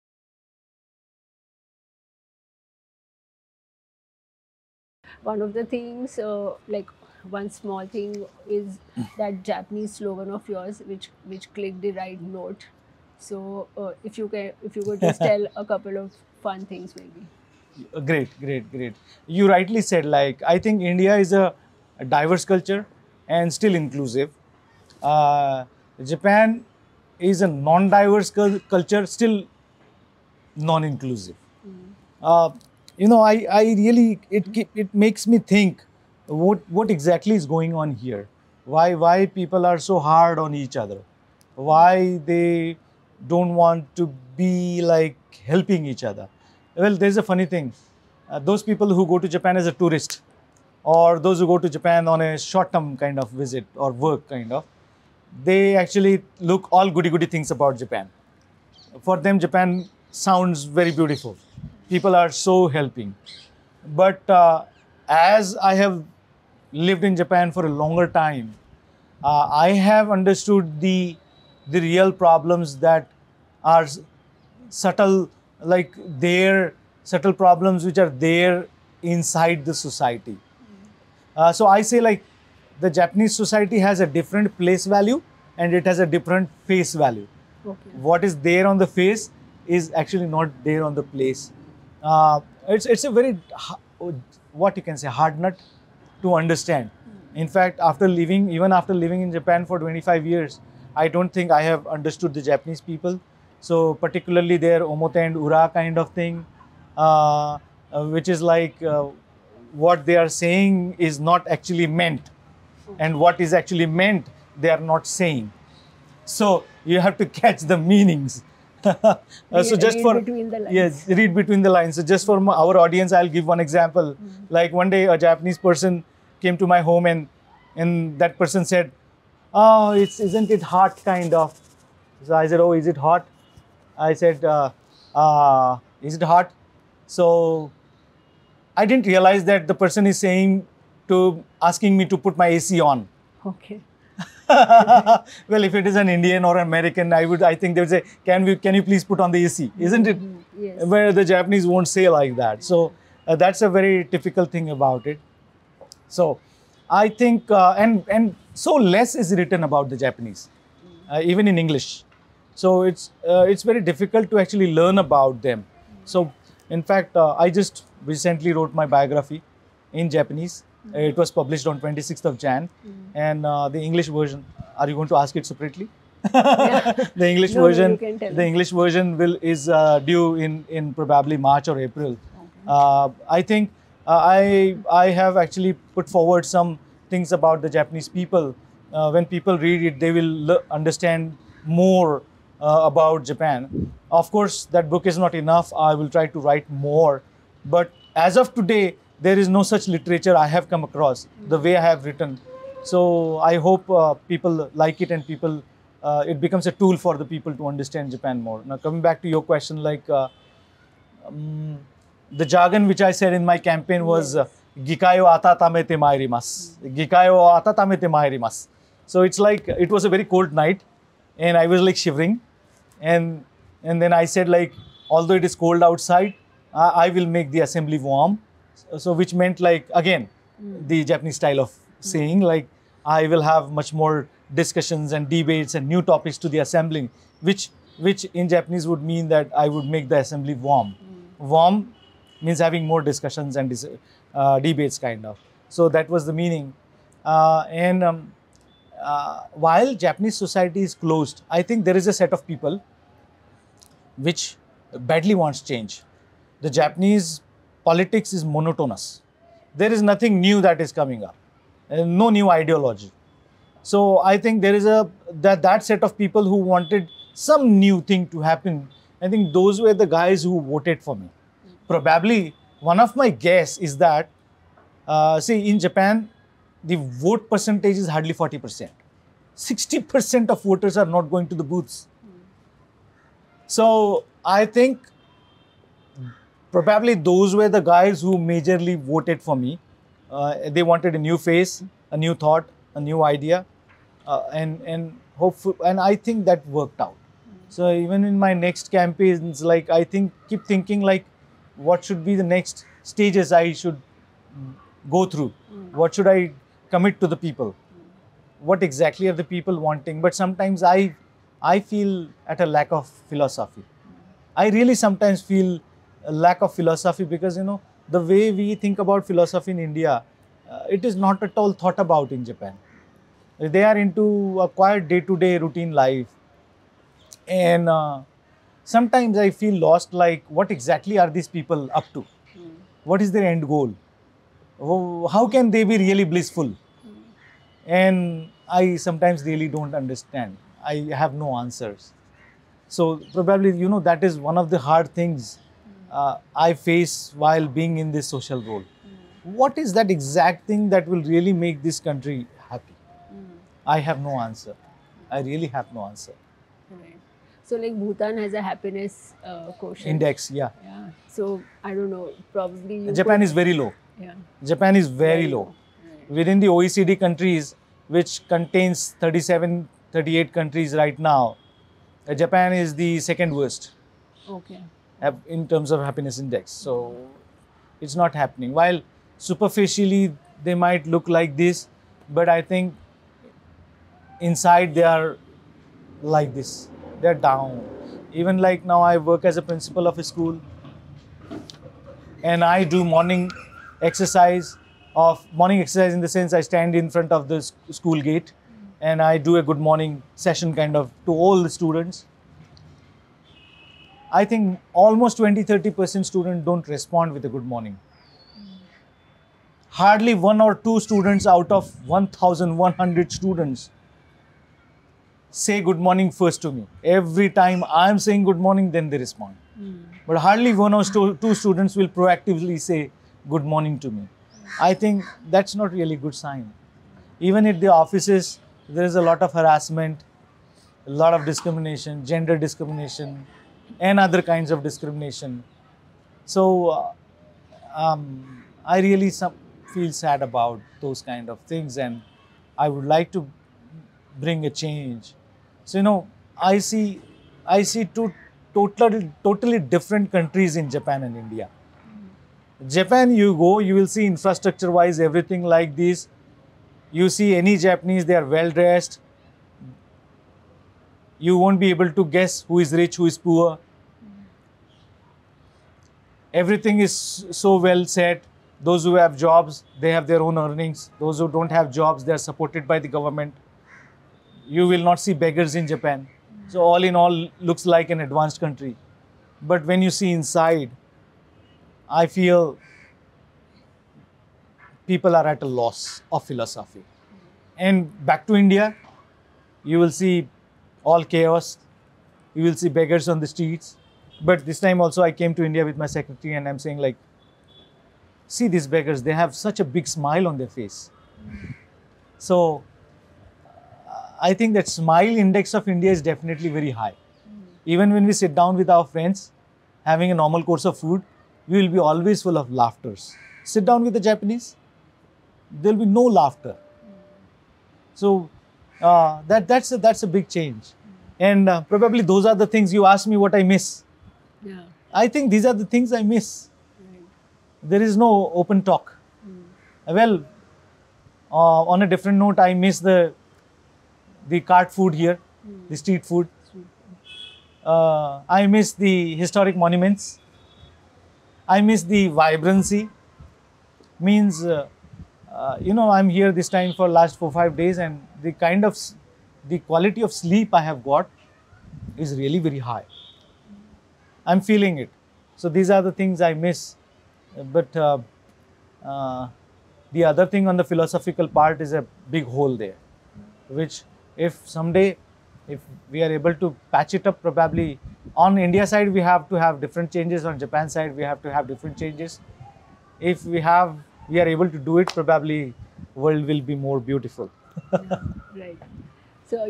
Leadership in Action - Mr. Yogendra Puranik, 1st Asian origin elected leader in Japan.
His views on Japan & the Japanese - how they have differing place & face values..